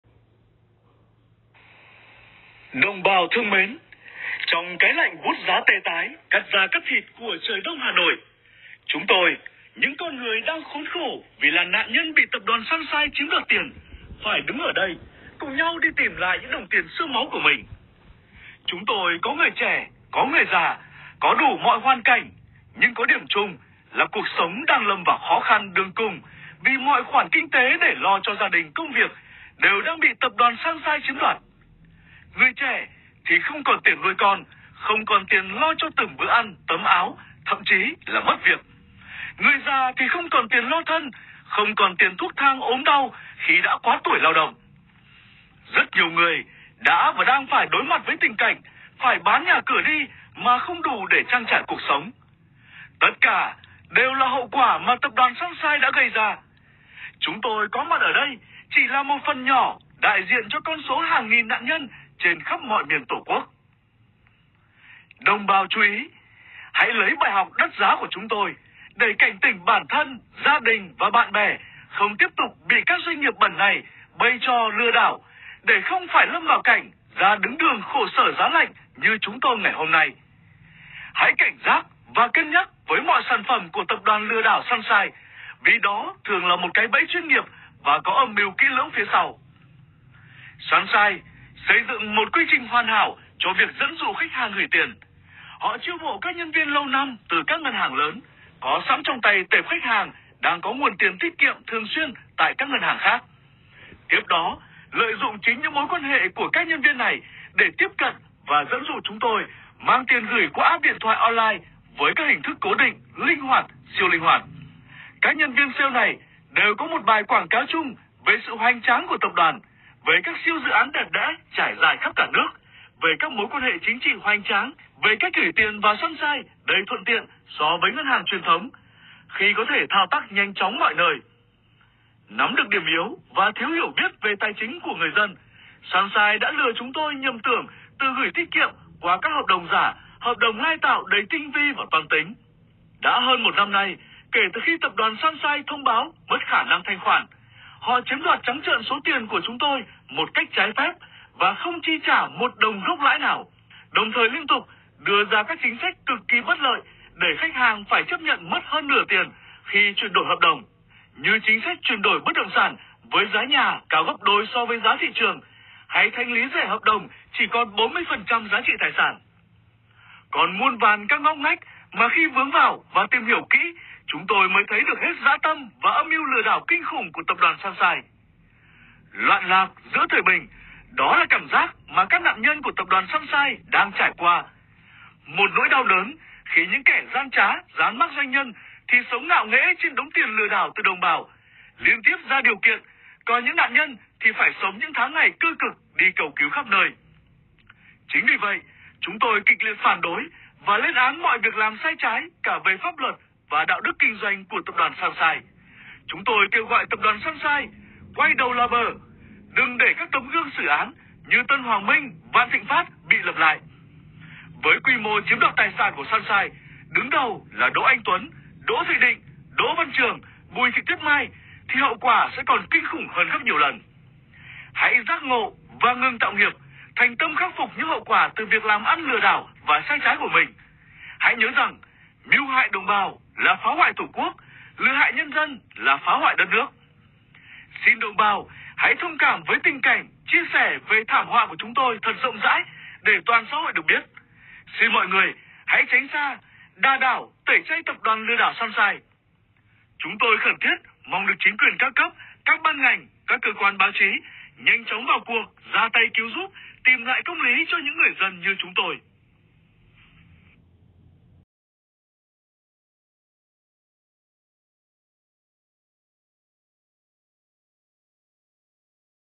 Chủ tịch tập đoàn Sunshine Group Đỗ Anh Tuấn mang tiếng là doanh nhân nhưng phải sống chui sống lủi
Chủ tịch tập đoàn Sunshine Group Đỗ Anh Tuấn mang tiếng là một doanh nhân mà sống chui sống lủi như một con chú cụt đuôi.
Hơn một năm nay ko trả tiền cho những người gửi tiền tiết kiệm linh hoạt, gửi trái phiếu, gửi đầu tư Bất động sản.
Thật là xấu hổ cho một doanh nhân ẩn bẩn, ở bẩn, phải luồn cúi khắp mọi nơi.